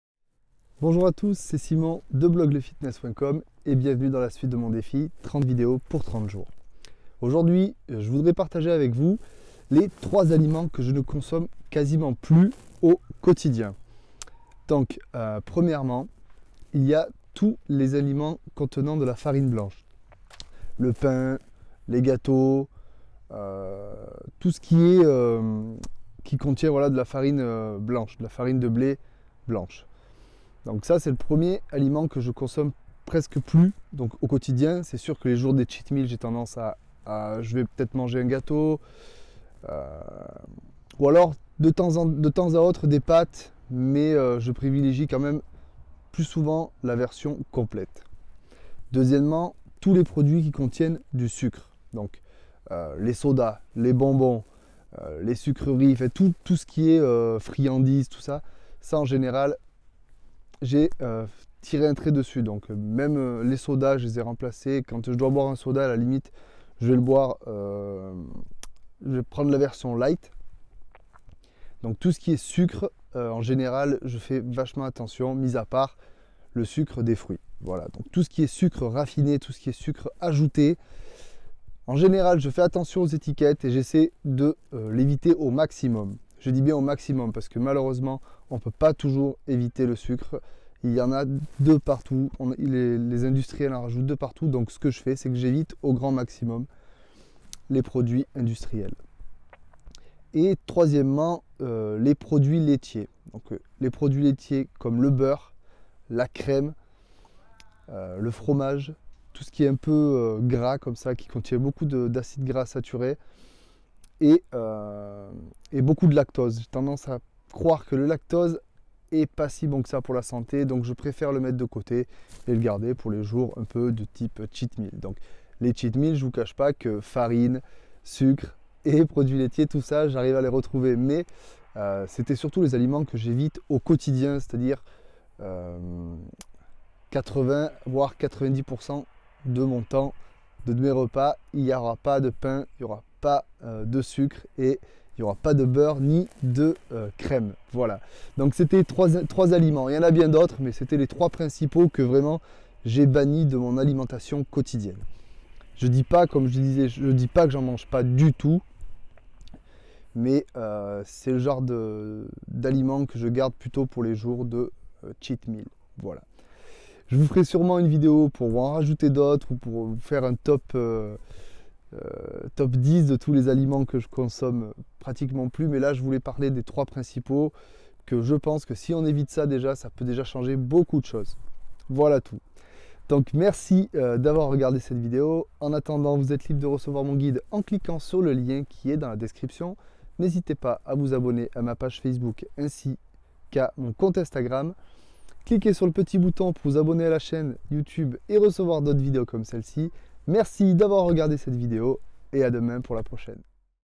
3 aliments que je ne consomme plus au quotidien (24/30)
Dans cette vidéo, je partage avec les 3 principaux aliments que je ne consomme quasiment plus au quotidien.

En complément de cette vidéo, vous êtes libre de recevoir mon guide "Les 3 secrets pour se muscler" en cliquant